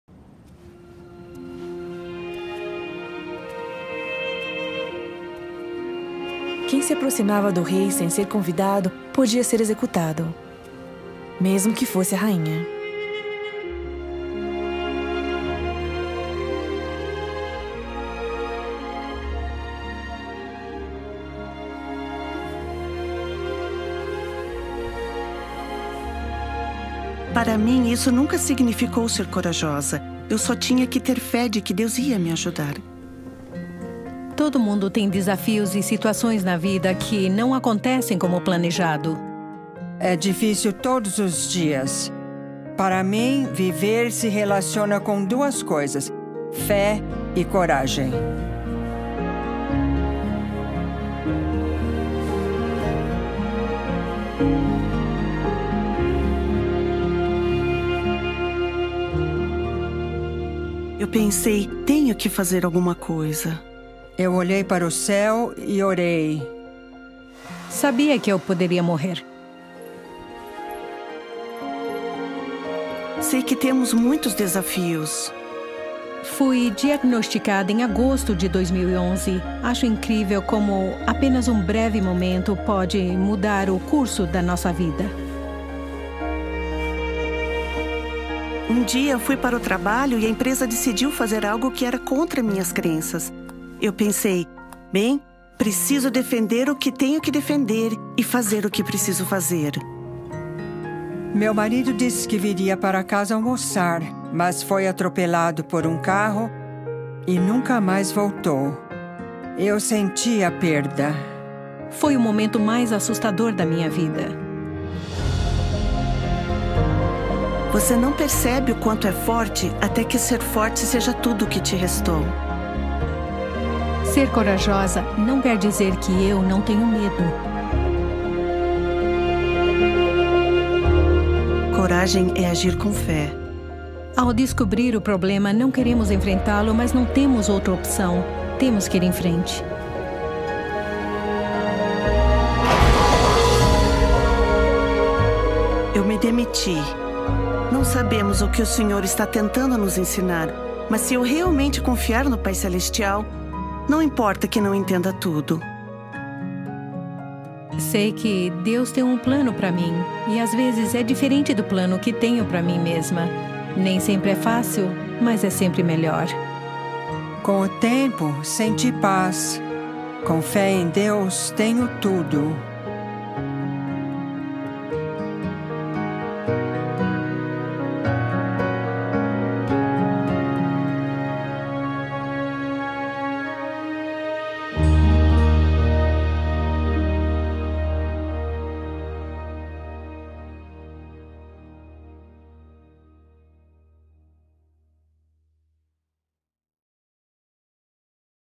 Coragem
Aprenda com a vida de Ester e de outras mulheres inspiradoras a importância de confiar em Deus e defender com coragem o que você acredita.